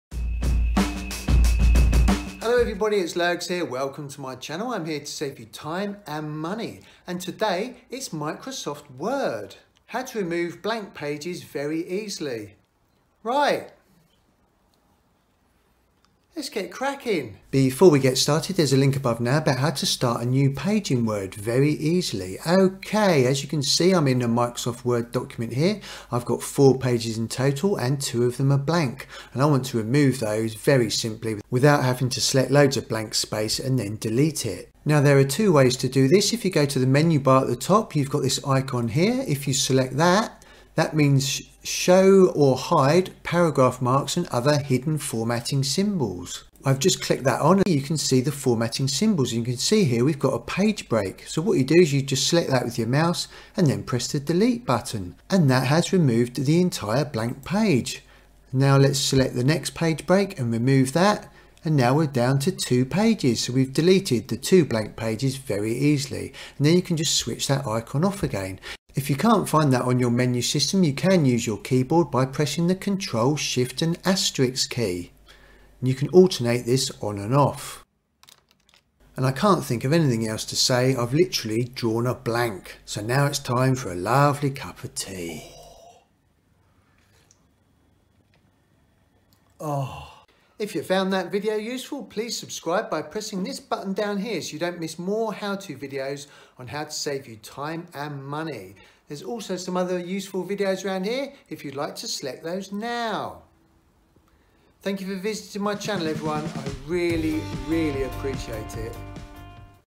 How to remove Blank Pages in Microsoft Word Easily
In this video I will show you how to easily delete blank pages in Microsoft Word.  If you print then you don't want to be printing blank pieces of paper.
How to remove blank pages in MS Word is an easy task.